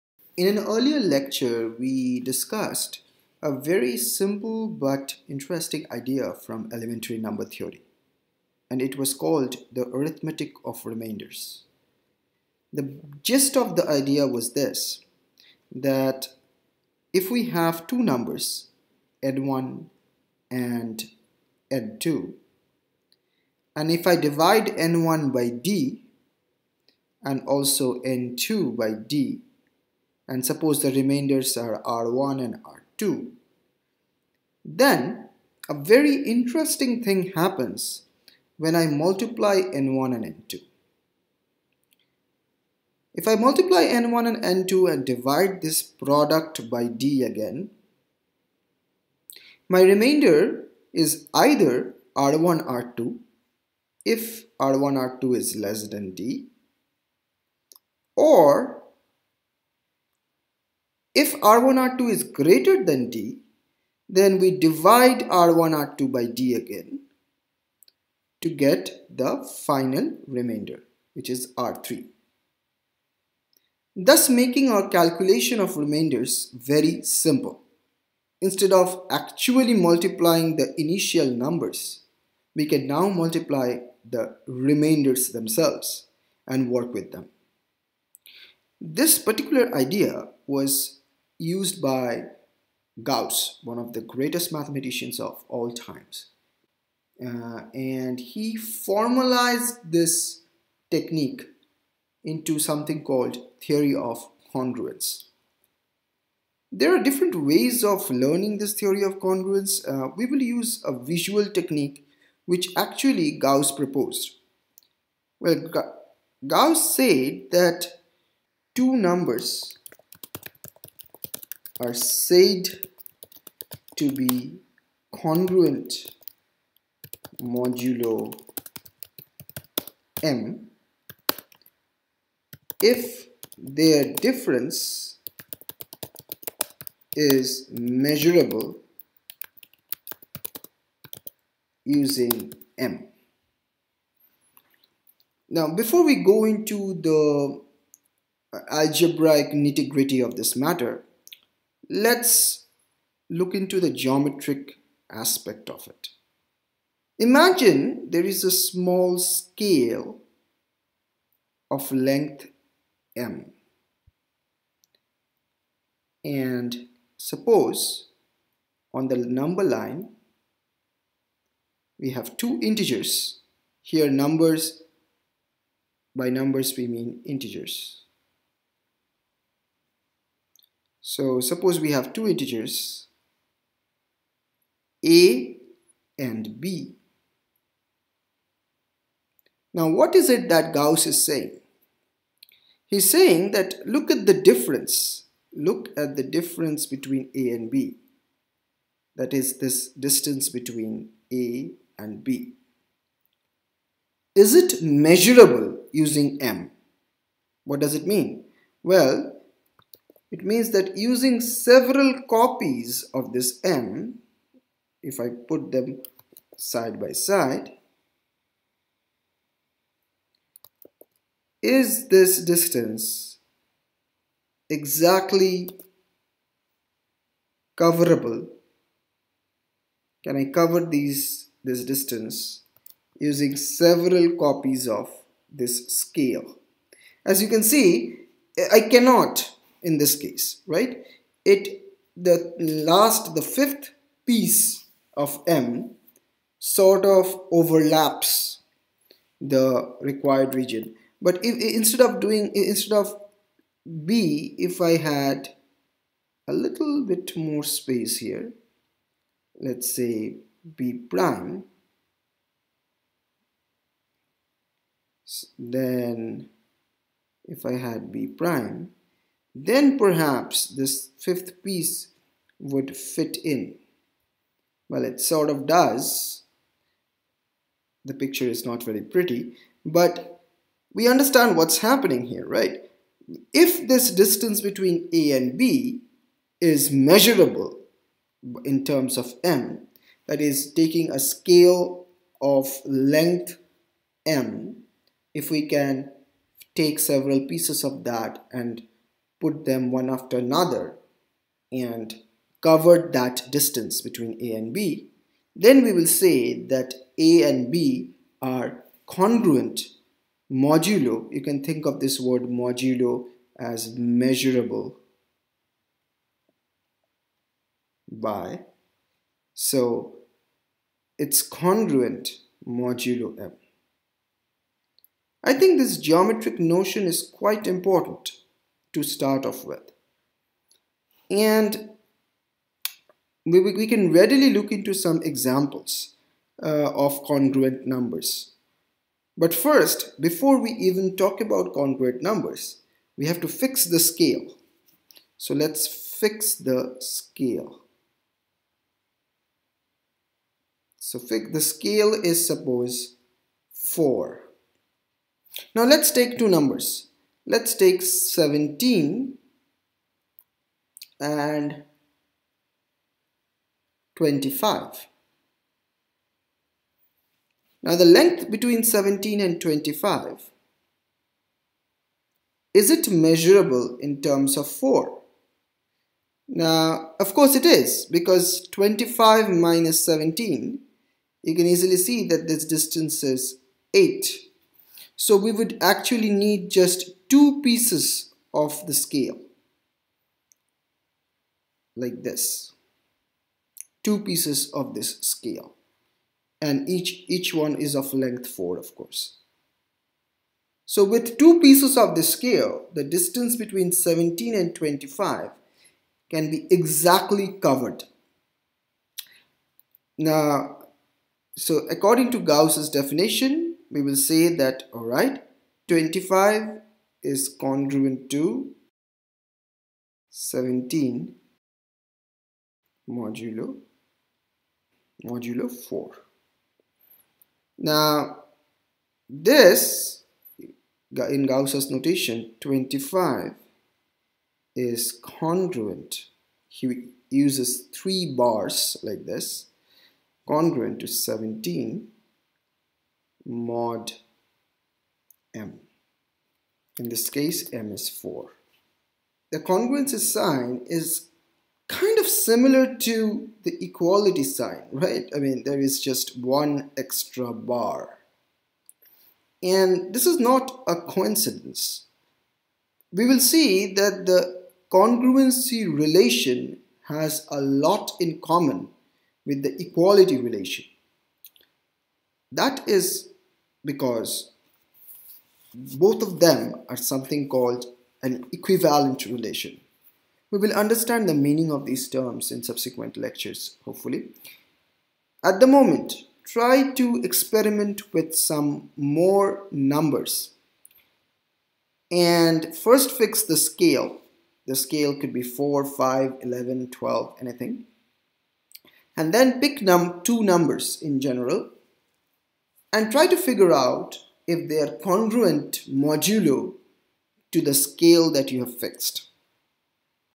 Theory of Congruence | Introduction with Geometry
In this video, we are going to discuss theory of congrueny, which is a continuation of the previous video.

Happy learning!